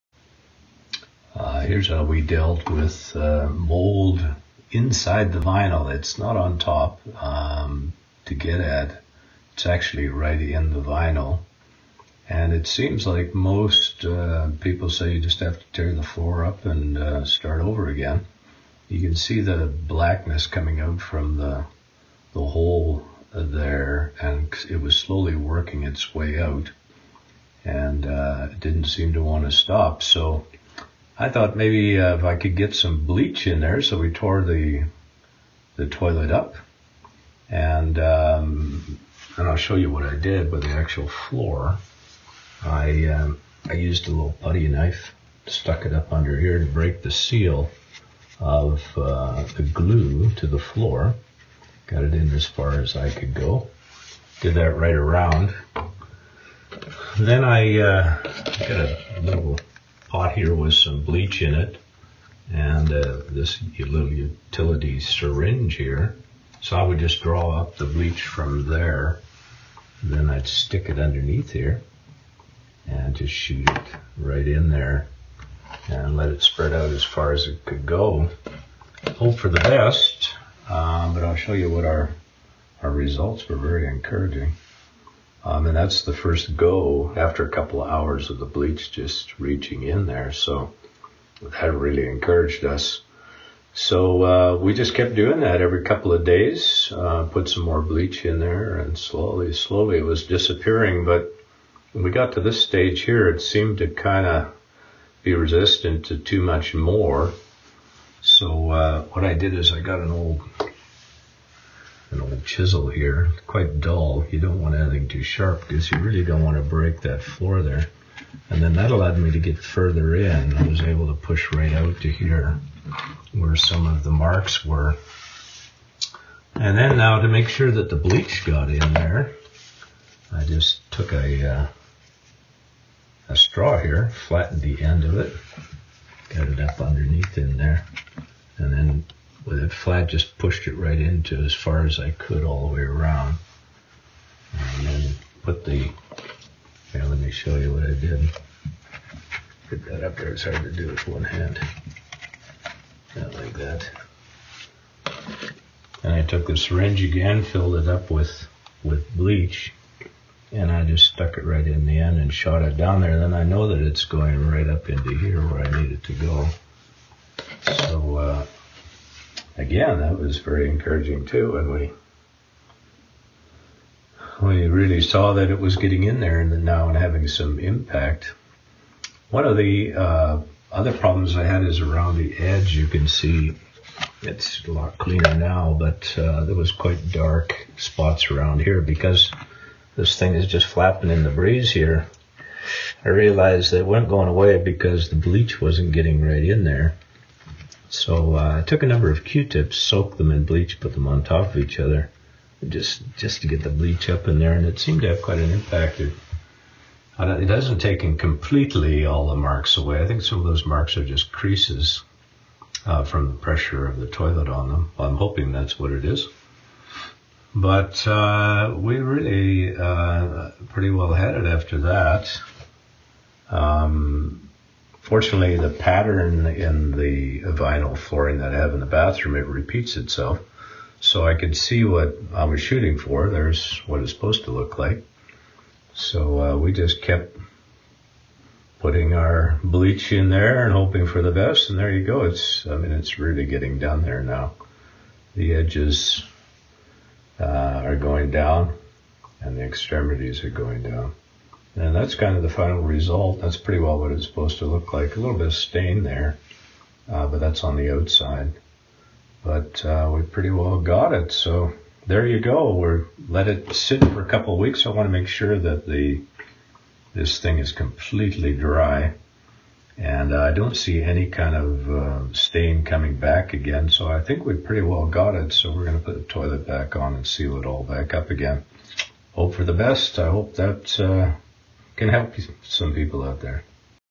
How To Clean Embedded Mold In Linoleum Flooring| HandyGrandpaIvor
In this video I will show you how I cleaned mold out of the layers of my bathroom linoleum flooring.